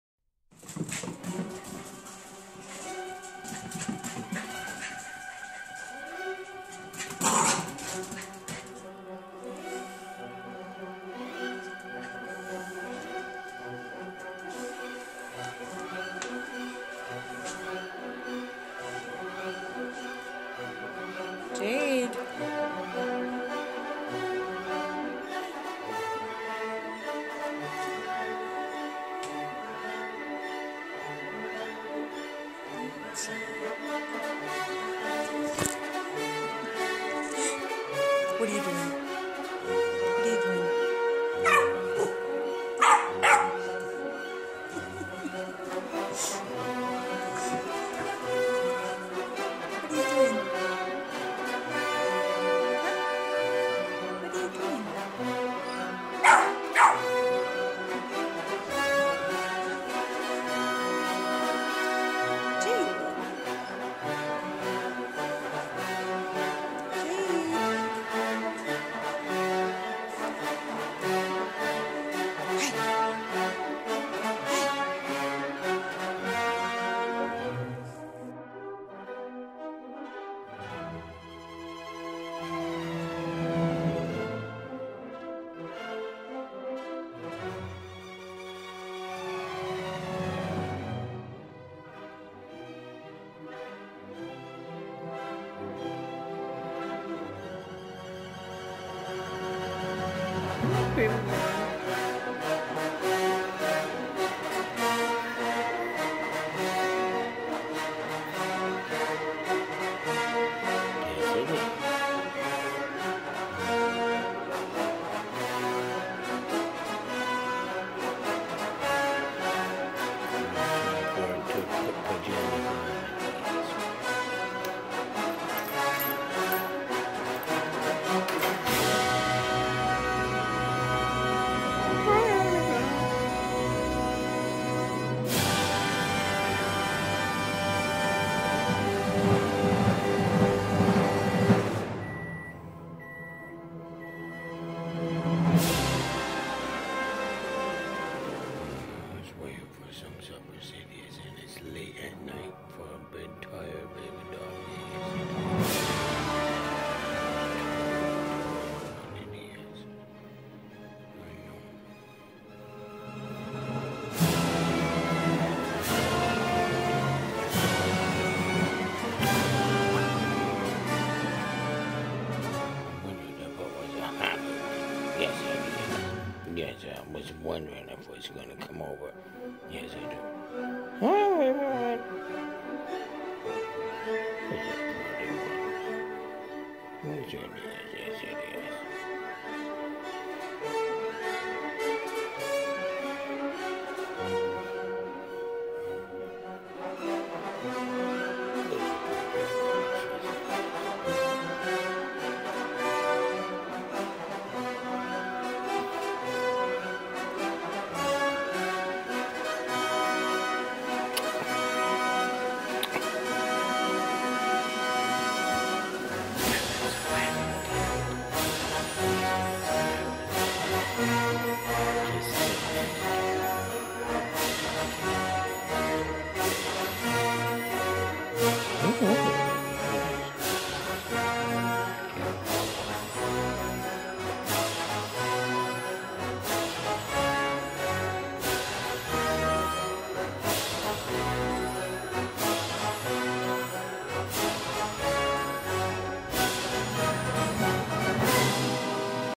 First Movie with babies
ALL beautiful Baby Doggies, Francesca, Barbie, and ONYX.

REGAL, REGAL, and super REGAL is all I can say. Respect the babies ! PLEASE. They listen as we should "whisper".